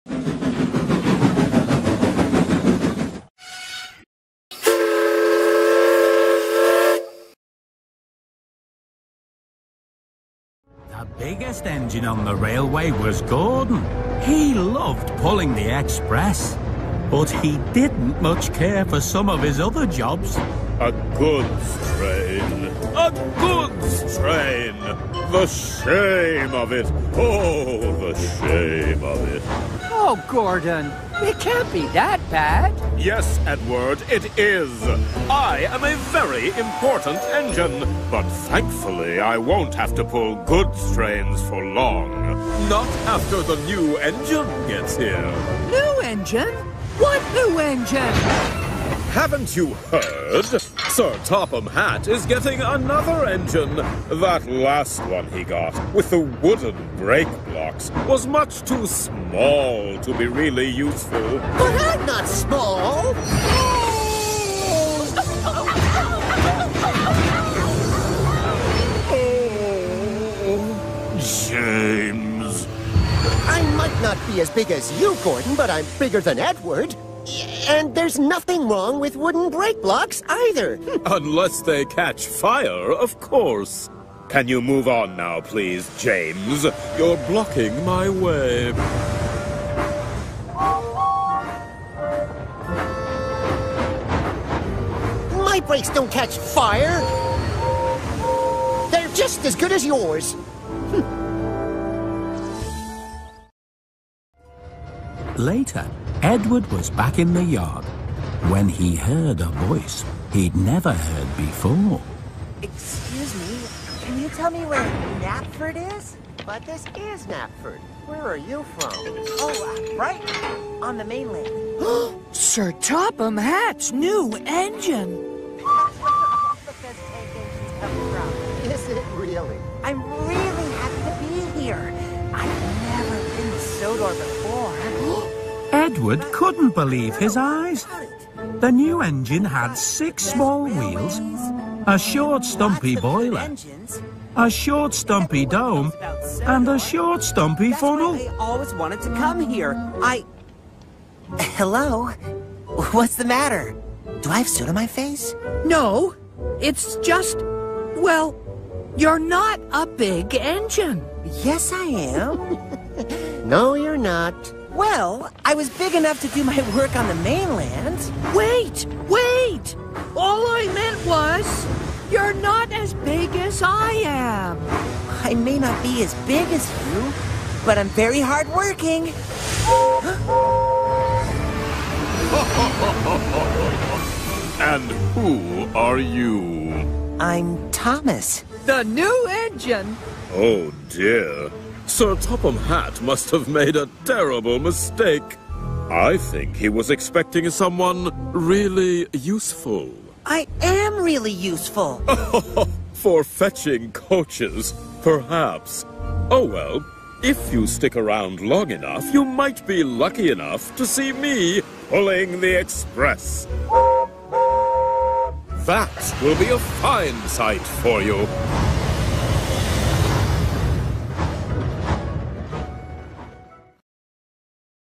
New Engine 4
Today, we welcome two new engines and one truck. Thomas and Friends is owned by Mattel Creations.